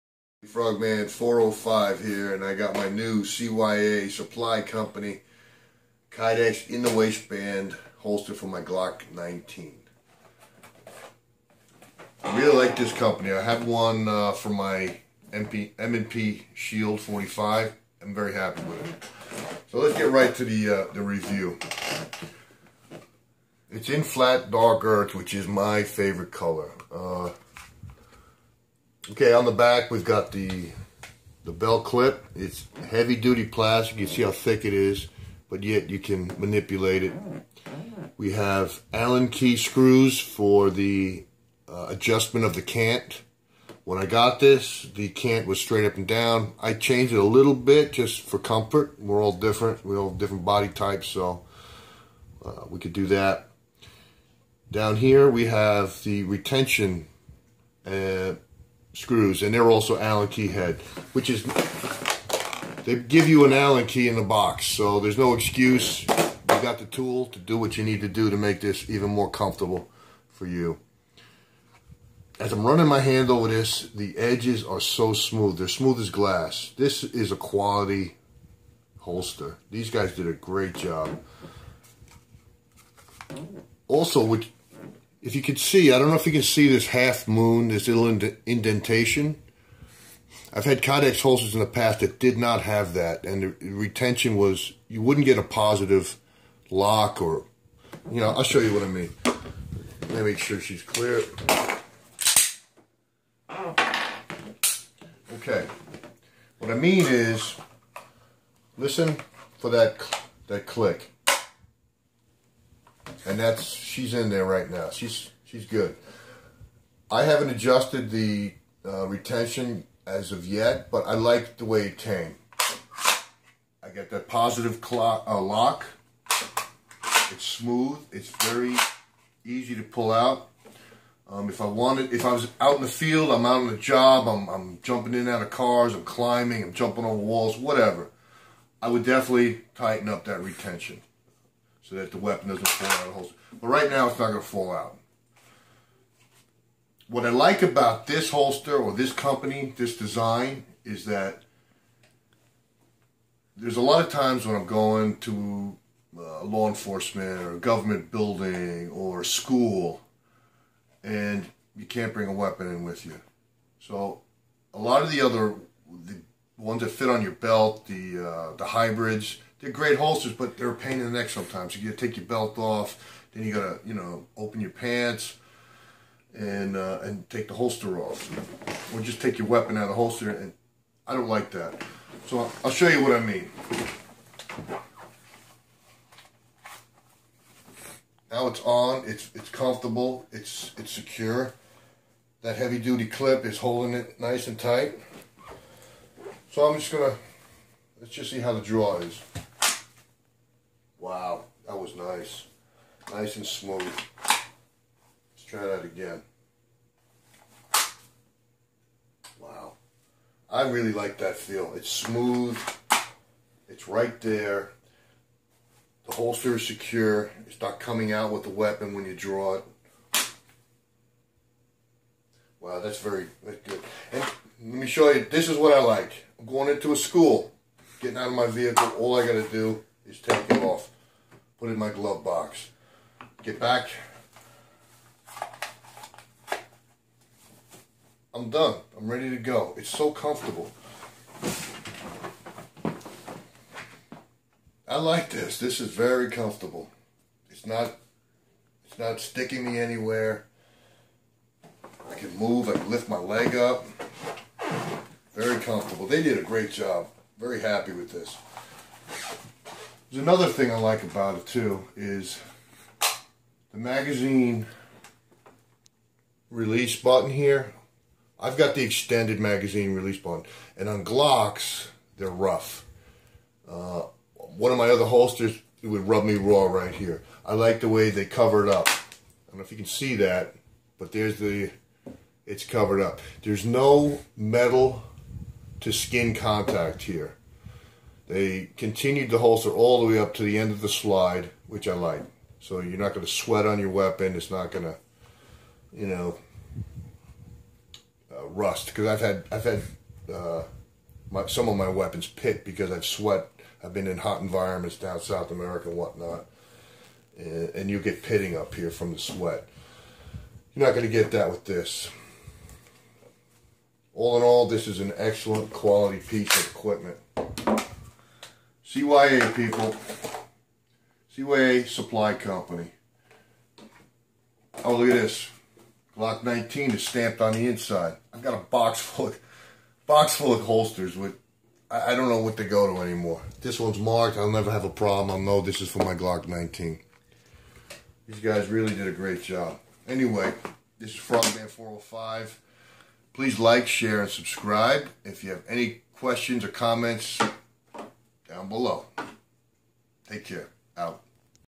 Part 2 Great AIWB holster for Glock 19 for the money..
In-depth review of the cya appendix in the waistband Glock 19 holster.